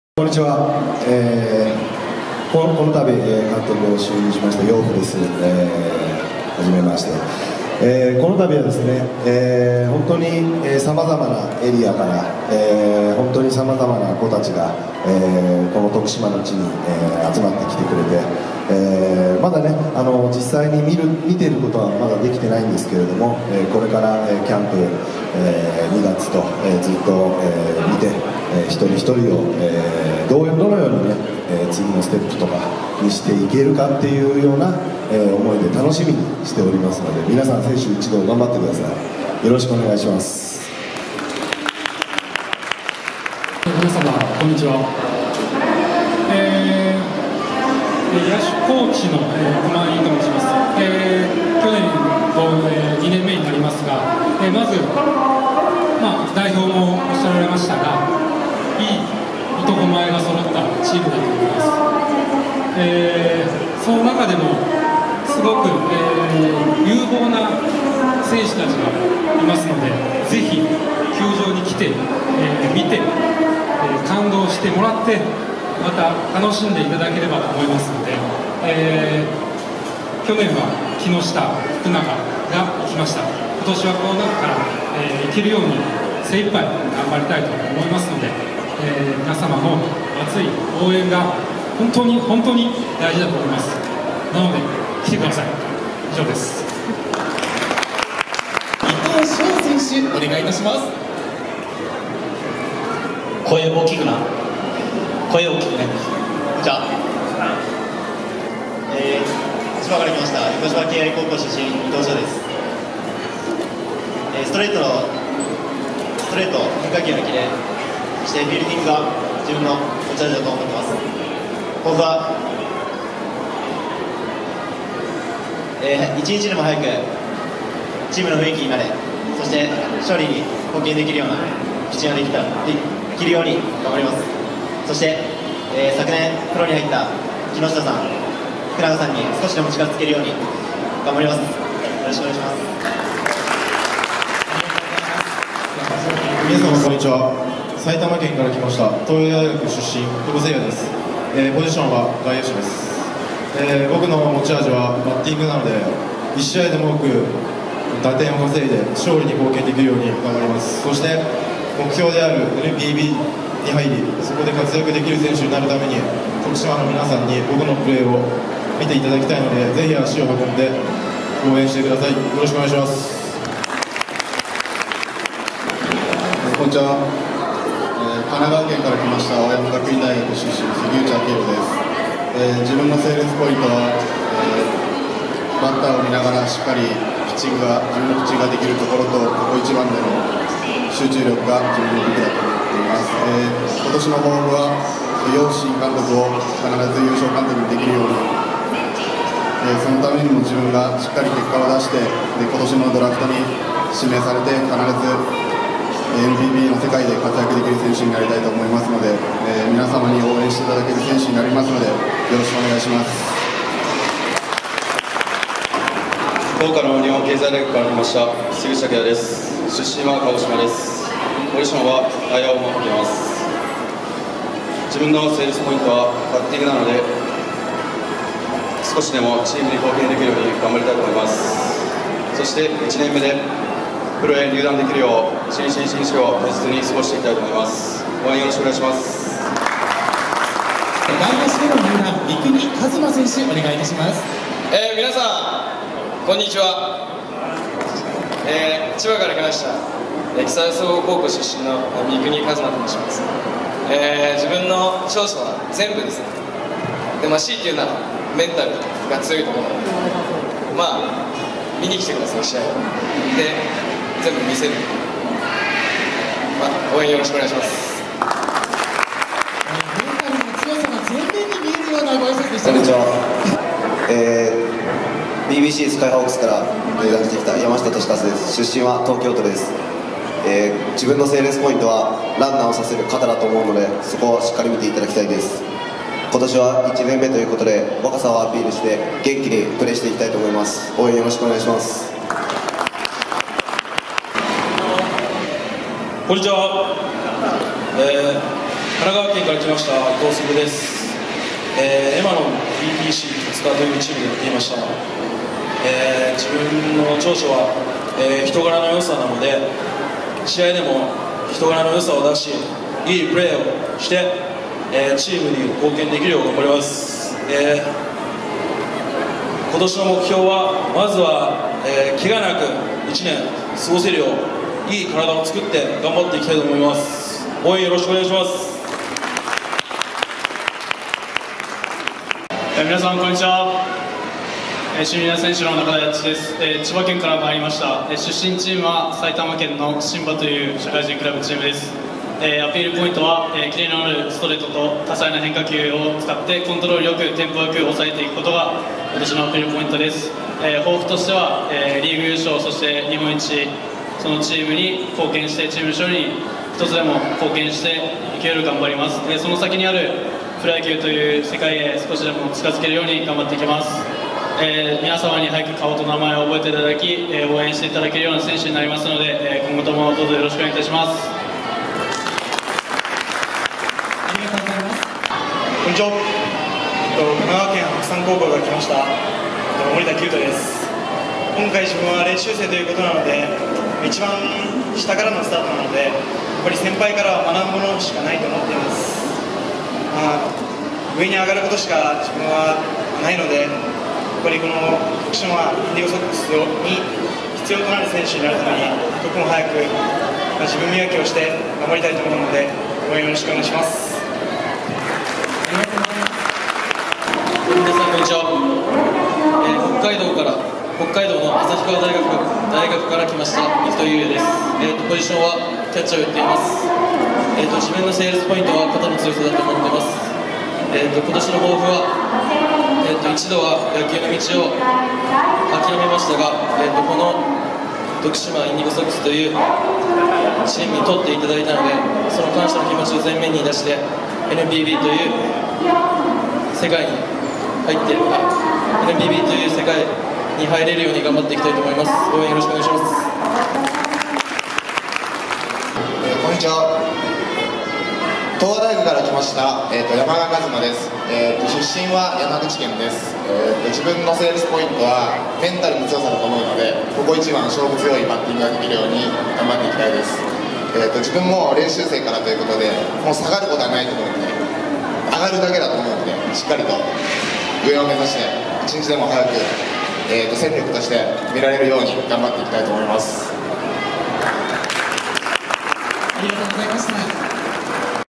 2017年度 徳島インディゴソックス 新入団選手発表記者会見（2017/01/28）
2017/01/28
ゆめタウン徳島
徳島インディゴソックス 新入団選手発表